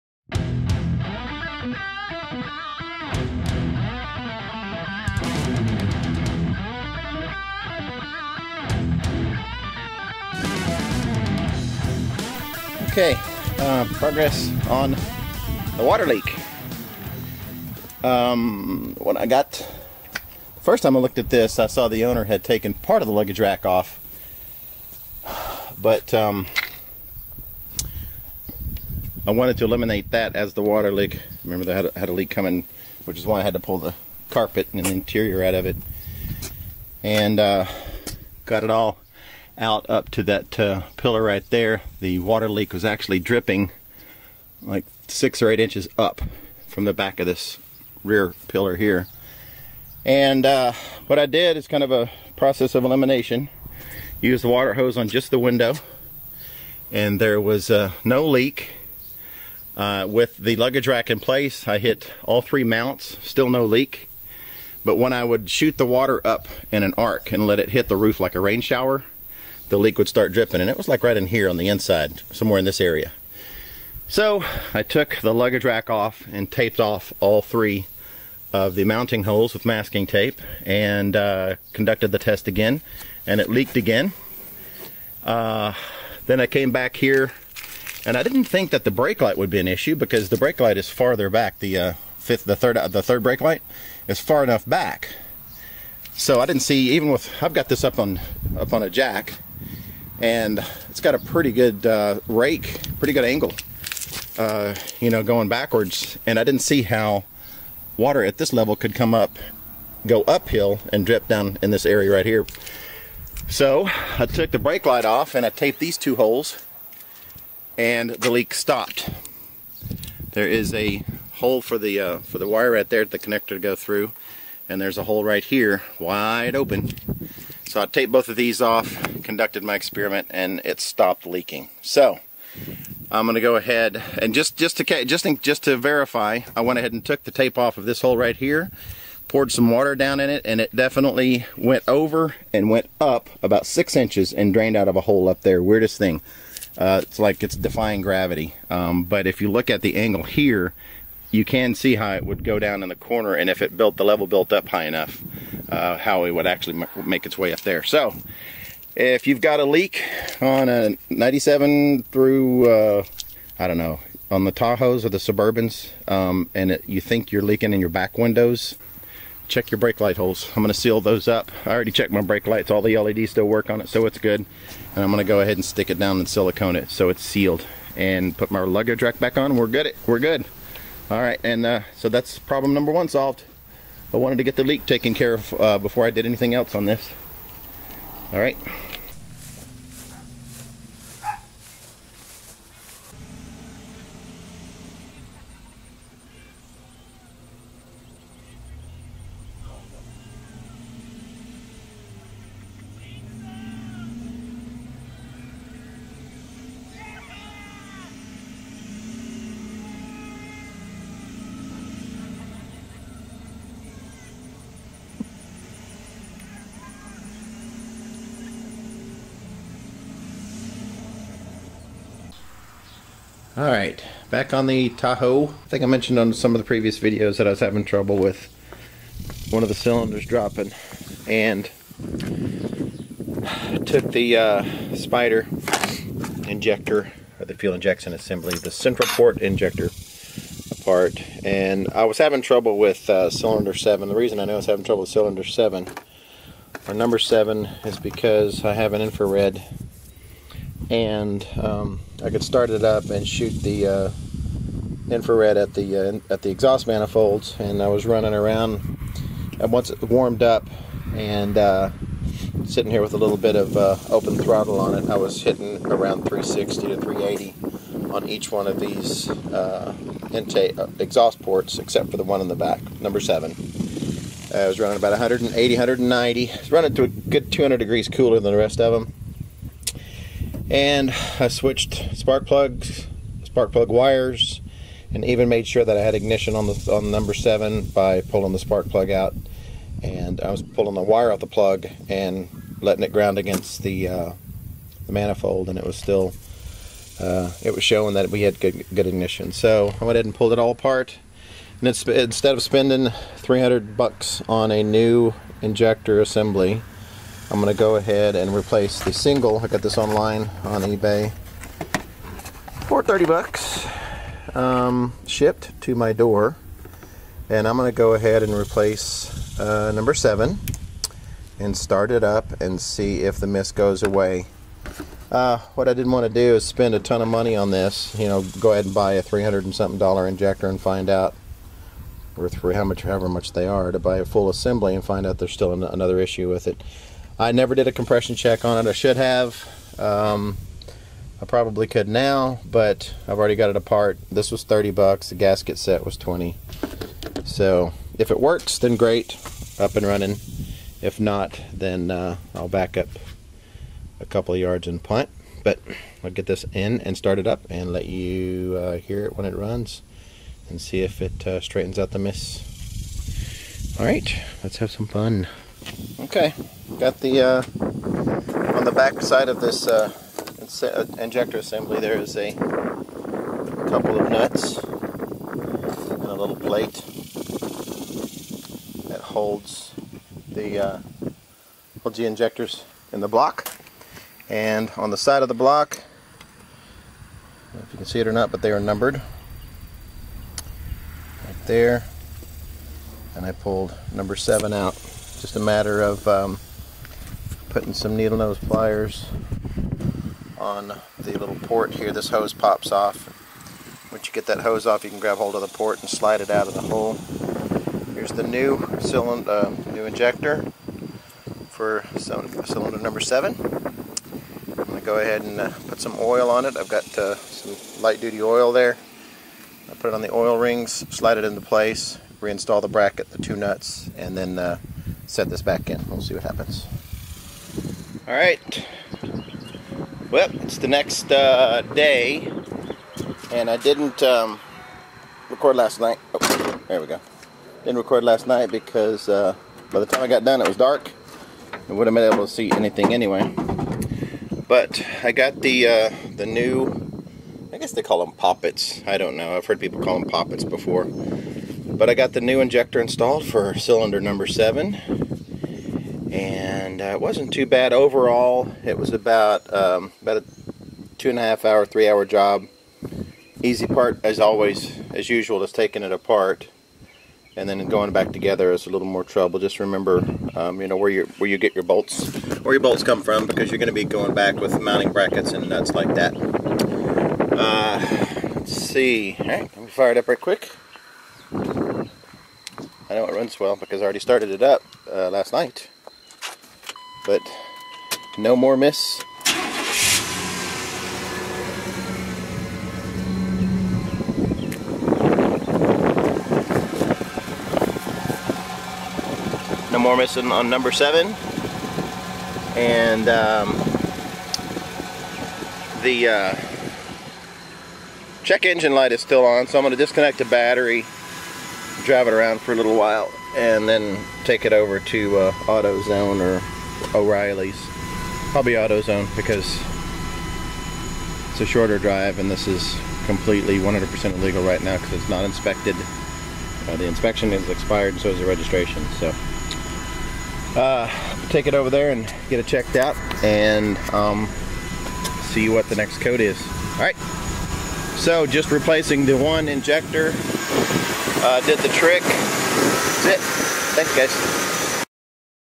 97 Tahoe Part 3- Update 1 Roof Leak and New Injector
Well, I wasn't looking for it but I found a 1997 Chevy Tahoe- Mechanic's Special. Owner had almost $1000 in it for a fuel pump and water pump and had a miss on one cylinder that two shops couldn't find, according to him. I took it for a test drive and aside from the miss, it drove pretty good for a truck with 208k miles on it. He didn't want to spend another dollar on it so I bought it for $1,100.00.

Body is in great shape, paint is okay, couple of bad spots, A/C works but it needed the following from what I could tell initially:
*Tires
*Front End Rebuild
*Find the miss
*Driver's side power seat slider
*Driver's side window regulator
*Major water leak in the back (carpet was soaked)
*New bulbs in the back (brake, turn, stop)
*Passenger's side front door handle
"Service Engine Soon light (throwing 12 different codes, that's gonna be fun)
*Rear bumper adjusted (got rear-ended once, not bad but knocked the bumper down)
*Windshield wipers
*State safety and emissions inspection
*State registration
*Detailing
*Seat Covers, front and back
*Plastic trim for the dash including a new dash
*Front brakes and rotors (possibly rear shoes and drums as well)
*All fluids
*And I'm sure there will be more once I get into it.